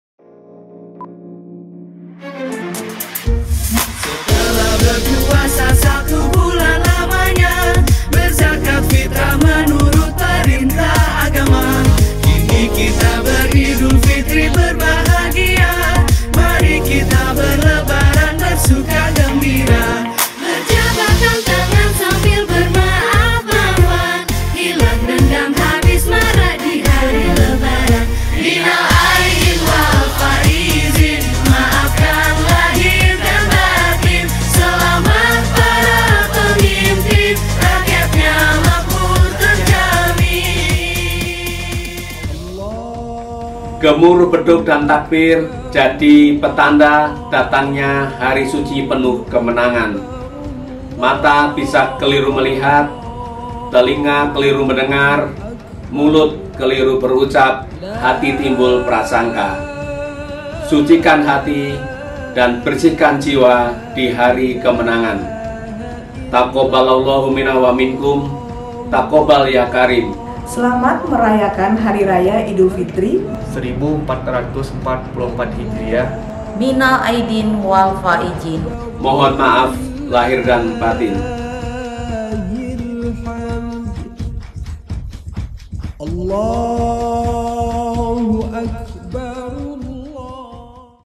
Selamat Idulfitri 1444 H, Minal Aidin Wal Faidzin
Seluruh jajaran pimpinan dan pegawai Balai Besar Uji Standar Karantina Pertanian (BBUSKP) mengucapkan selamat merayakan Idulfitri 1444 H. Minal aidin wal faidzin, mohon maaf lahir dan batin. Selamat berkumpul bersama keluarga 😊